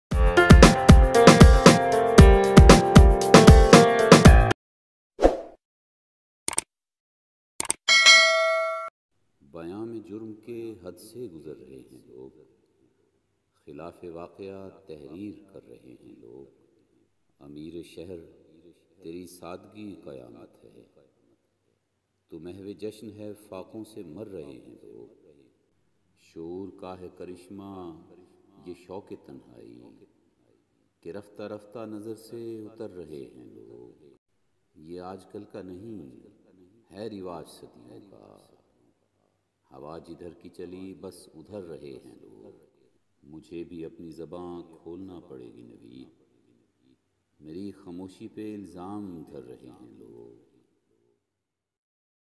شعور کاہے کرشمہ یہ شوق_تنہائی
بیاں میں جرم کے حد سے گزر رہے ہیں لوگ
خلافِ واقعہ تحریر کر رہے ہیں لوگ
شعور کا ہے کرشمہ یہ شوقِ تنہائی
کہ رفتہ رفتہ نظر سے اتر رہے ہیں لوگ
یہ آج کل کا نہیں ہے رواج صدیوں کا
ہوا جدھر کی چلی بس ادھر رہے ہیں لوگ
امیرِ شہر تری سادگی قیامت ہے
تُو محوِ جشن ہے فاقوں سے مر رہے ہیں لوگ
مجھے بھی اپنی زباں کھولنا پڑے گی نوید
مری خموشی پہ الزام دھر رہے ہیں لوگ
نویدصدیقی
People are crossing the limits in telling about the  crimes,
and are writing against the truth
This  solitude is a passion of consciousness
People are loosing their grace .
This is not a trend of recent times, it's been going on for centuries
Wherever the wind blows, people are there
Your simplicity, O wealthy of the city, is  so astonishing,
You are lost in celebration while people are dying in the streets
I too will have to speak up in my own language, Naveed
People are accusing my silence.